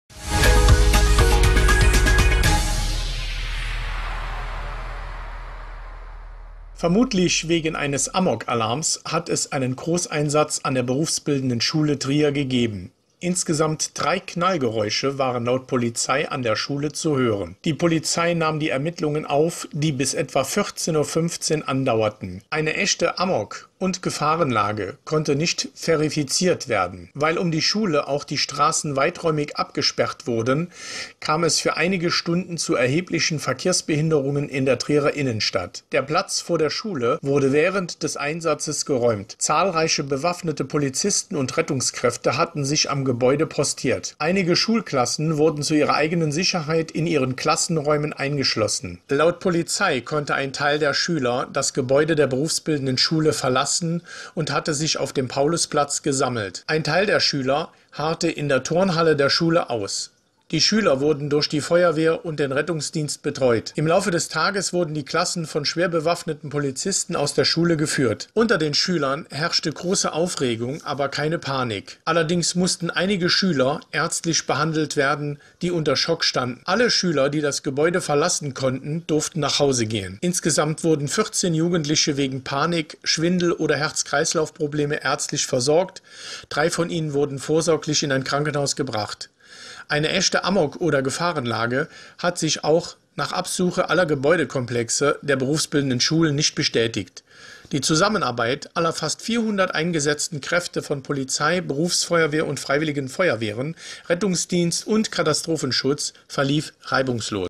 Fehlalarm: Laute Knallgeräusche lösen Amokalarm an der BBS Trier aus
Dreimal knallt es am Freitagmorgen in der Innenstadt. Die Geräusche, deren Ursprung bis jetzt unklar ist, lösen einen Amok-Fehlalarm aus, in dessen Verlauf alle Gebäude des Berufsschulzentrums am Paulusplatz von schwerbewaffneten Polizisten durchsucht und evakuiert werden. 400 Rettungskräfte sind im Einsatz, der Verkehr bricht zusammen.